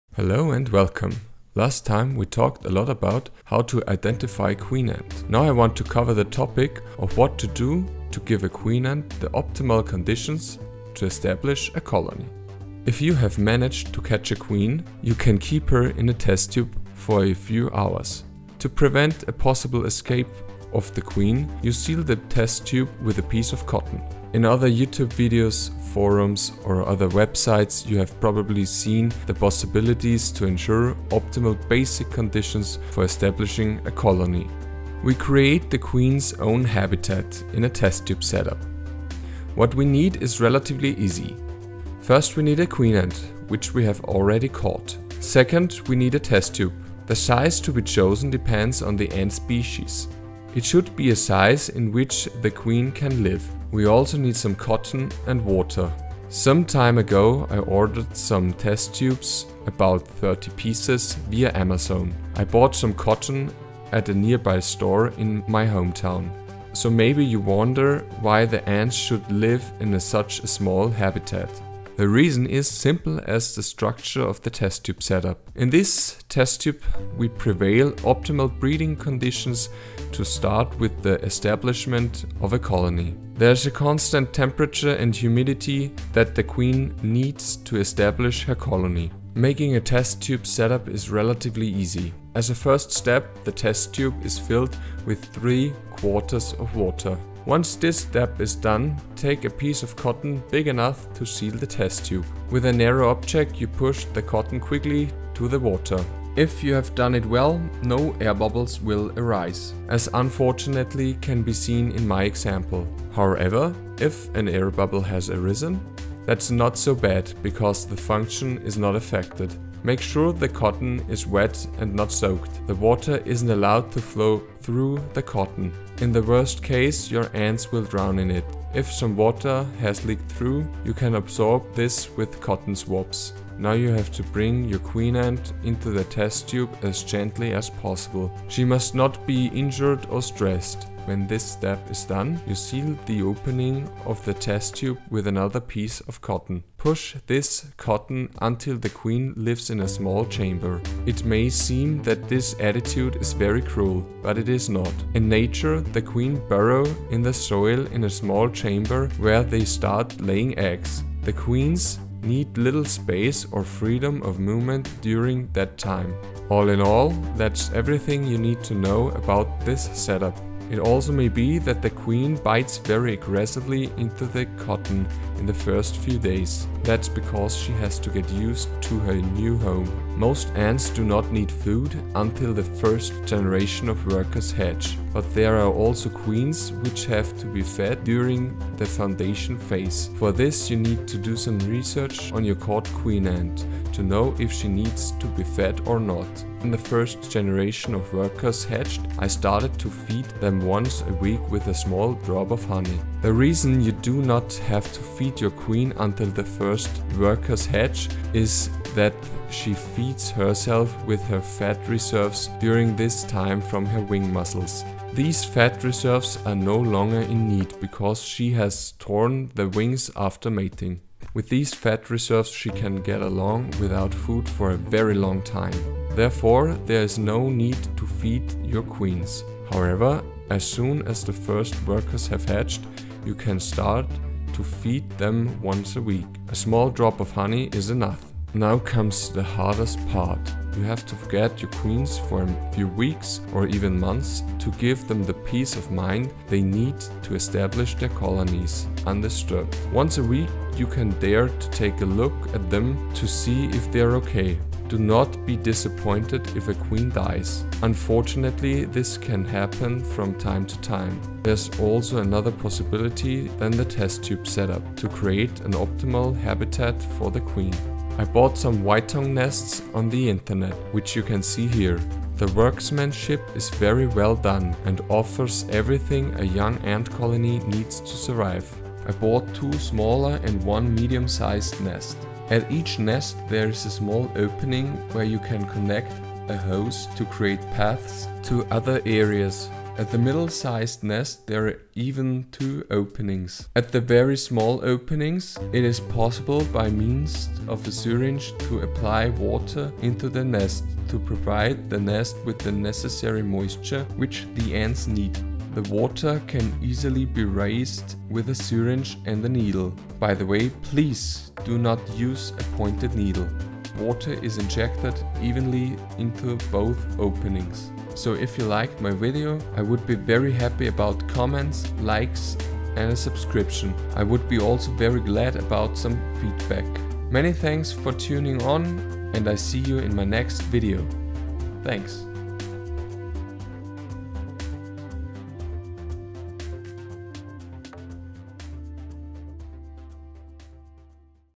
How to raise an ant colony?
Where can you nest your ant queen at the beginning of her ant colon? The best solution is a Test Tube Setup. In the video I explain the most important steps to accomplish this.